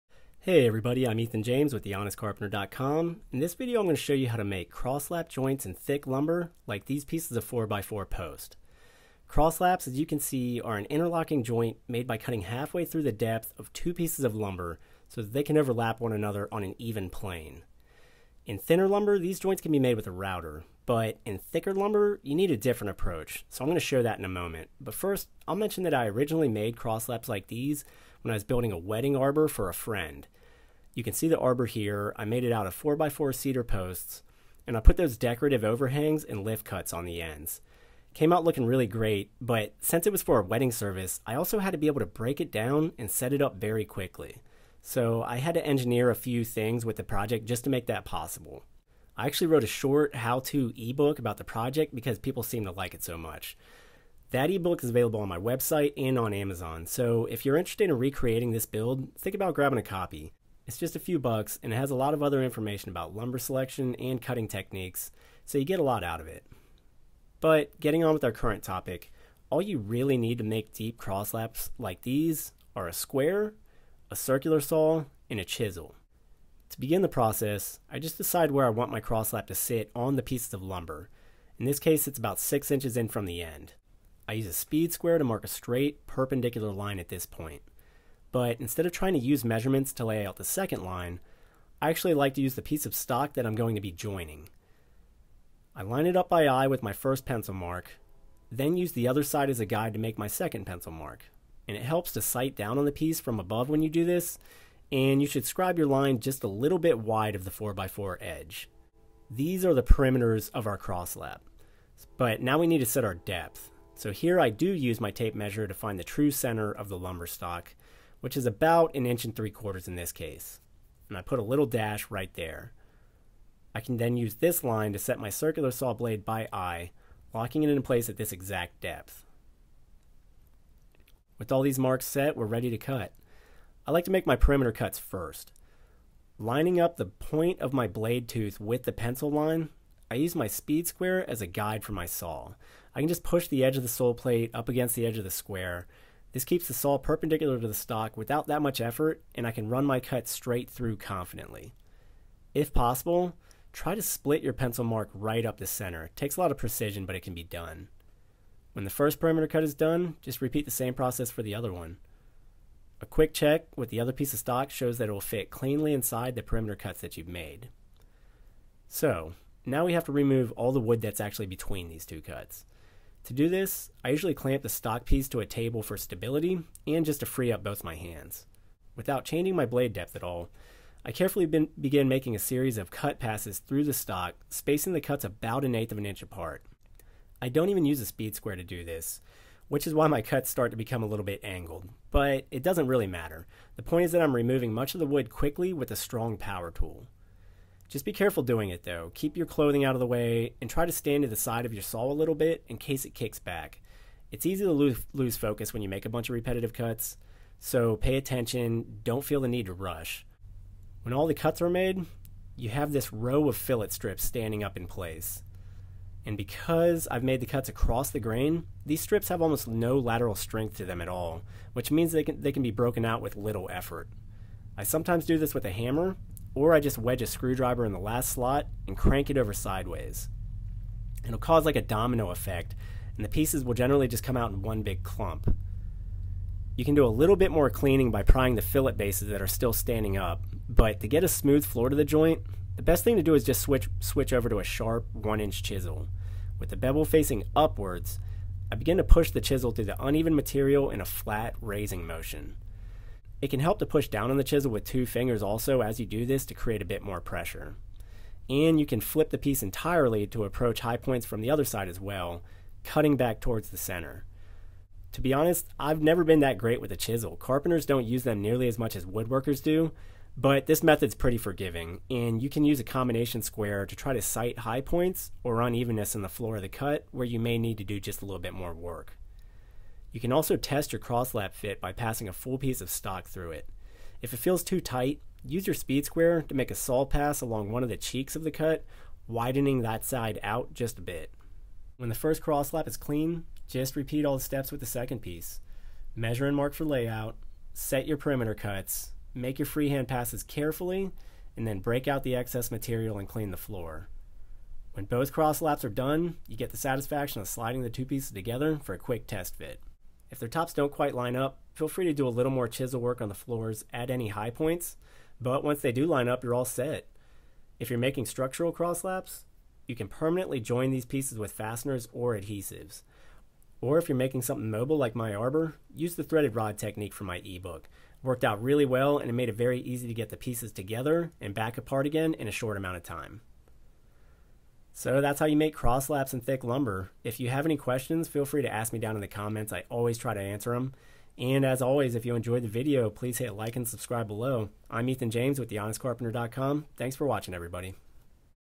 How To CUT CROSSLAPS (In Thick Lumber)
Stanley 3 pc Chisel Set:
VonHaus 10 pc Chisel Set:
Makita 7-1/4" Circular Saw:
Swanson Speed Square:

Crosslap joints allow you to bring two pieces of wood together on an even plane. This short video from The Honest Carpenter will show you how to cut crosslaps in thicker lumber like 4x4 posts!

And, here's an article on The Honest Carpenter website detailing this crosslap joint process (including tool links):

Tool List:
Tape Measure
Circular Saw
1" Chisel
Speed Square

How-To Cut Crosslaps:

1) Lay out the position of your crosslaps on the two pieces of lumber you would like to join. (Use a tape measure and speed square to create one side of your crosslap layout, then a piece of stock to scribe your other side.)
2) Set the depth of your cut by finding the center line of your lumber stock on the adjacent face of the material. Set your circular saw depth to this mark.
3) Use a speed square as a guide for your circular saw sole plate--cut the "perimeters" of your crosslaps. Try to split your pencil lines up the center.
4) Begin making cut passes through the stock between the perimeter cuts. Space cuts about 1/8" apart.
5) Break out standing fillets with a large screwdriver.
6) Clean the floor of the crosslap with a 1" chisel (keep bevel up)
7) Test fit with a piece of stock
8) Lay out and cut second crosslap on second piece.

Also, be sure to visit us at The Honest Carpenter website for more tips, articles and tool links: